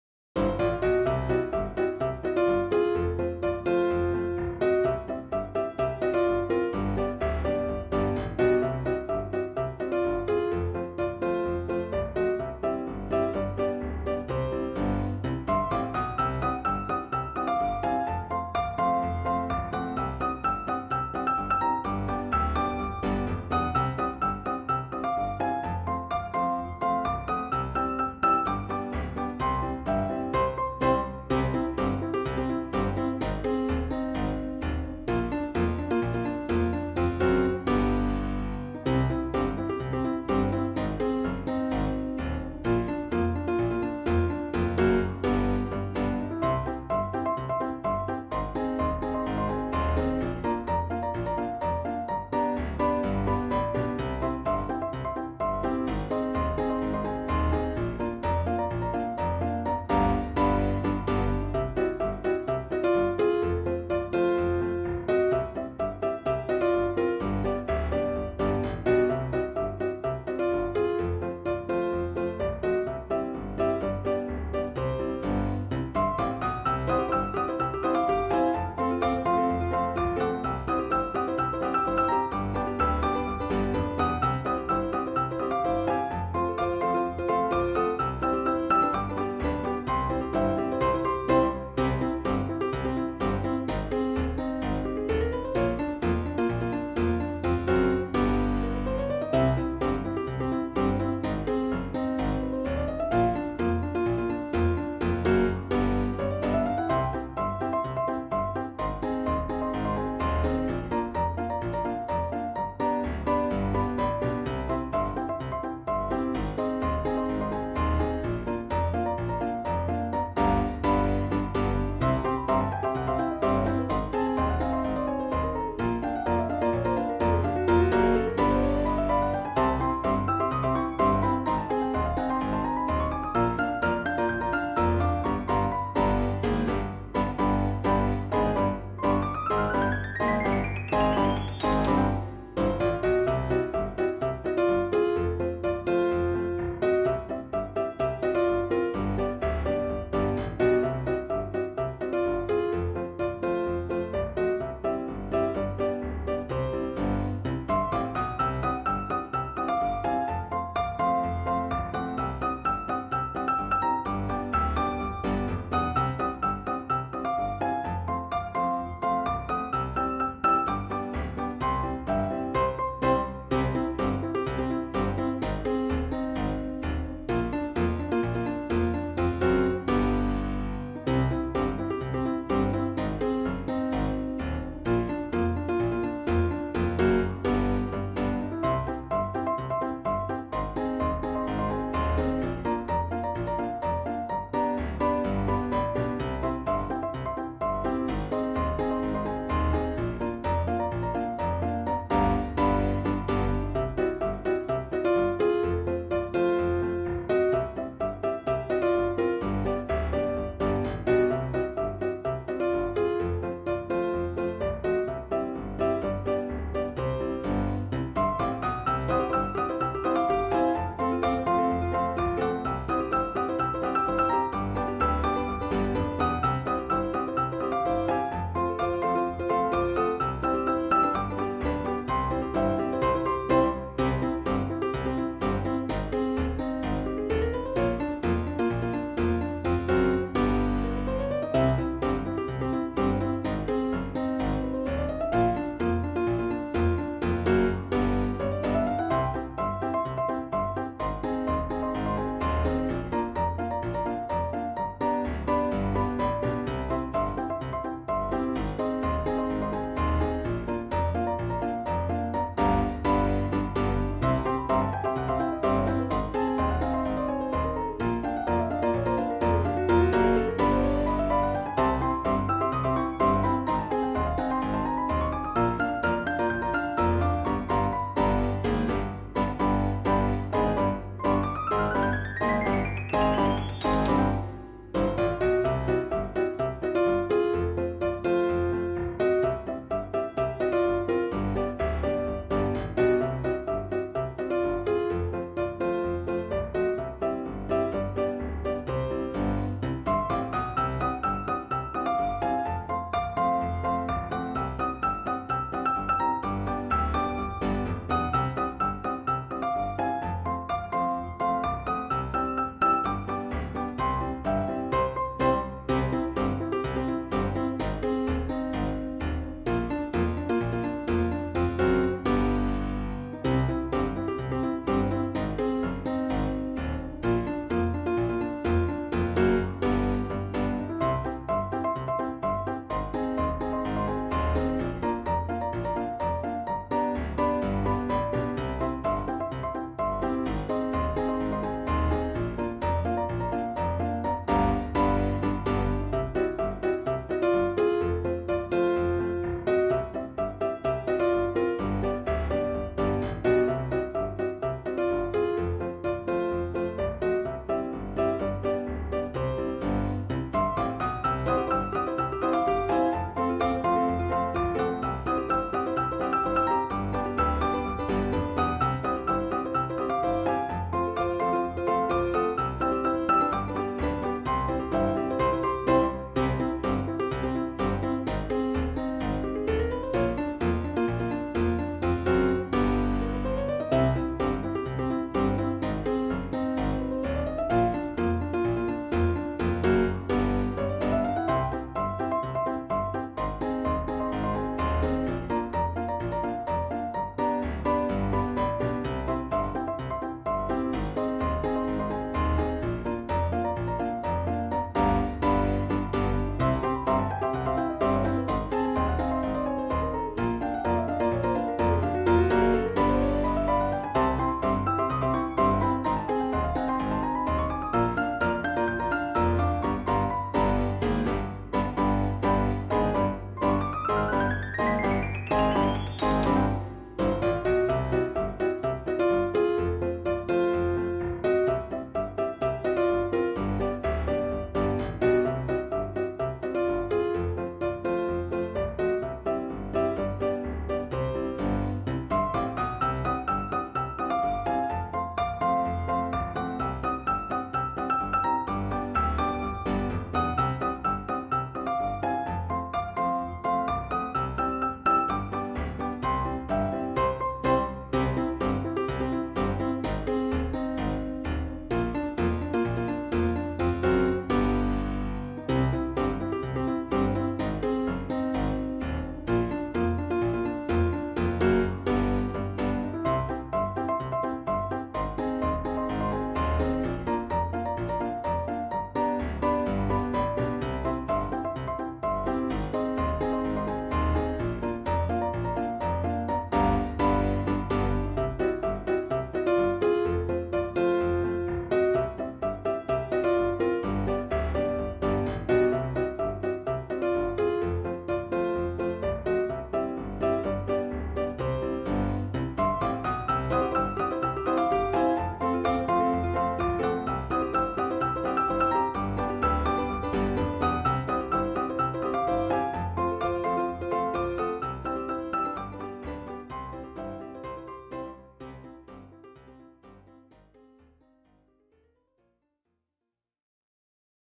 (Timelapse) Low-poly Traffic Light Game asset Modeling Texturing
Another Timelapse showing my workflow for Modeling in Houdini and Texturing in Substance Painter.